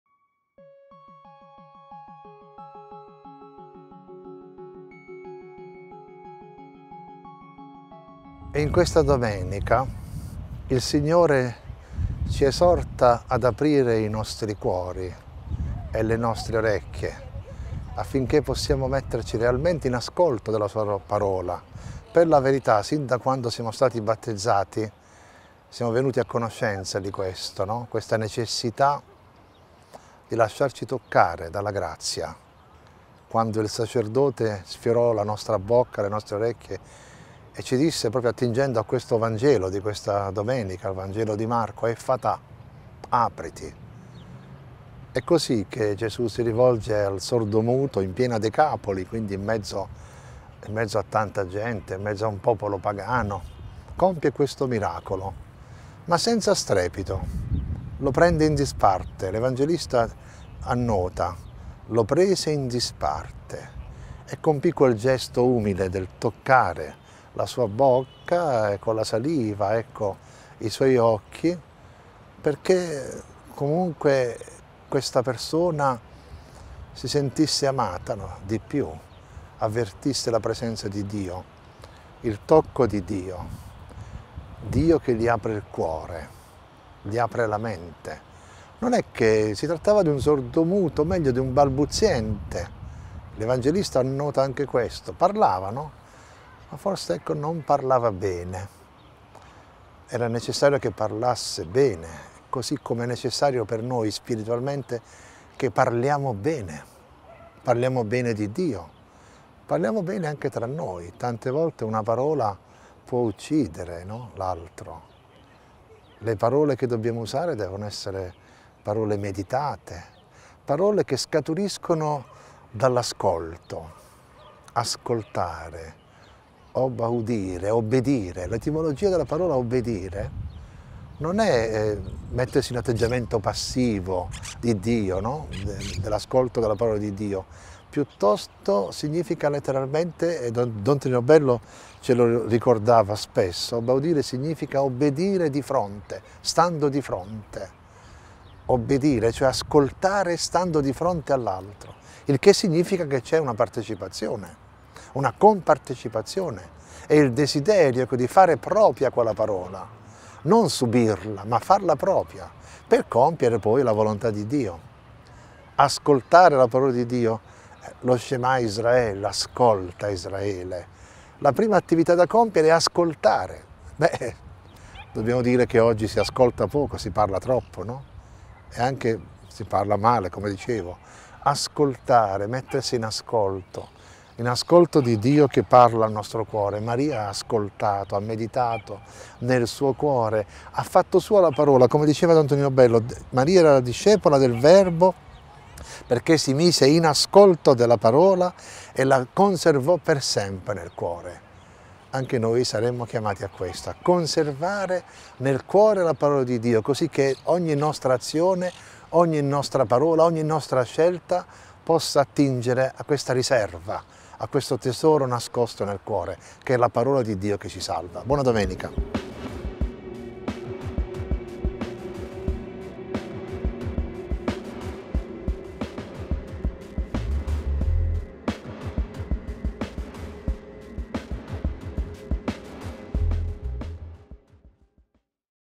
don Alfonso Giorgio - Commento al Vangelo del 5 Settembre 2021
Movimento Apostolico Ciechi
Riflessioni dell'assistente spirituale don Alfonso Giorgio per il Vangelo di domenica 5 Settembre 2021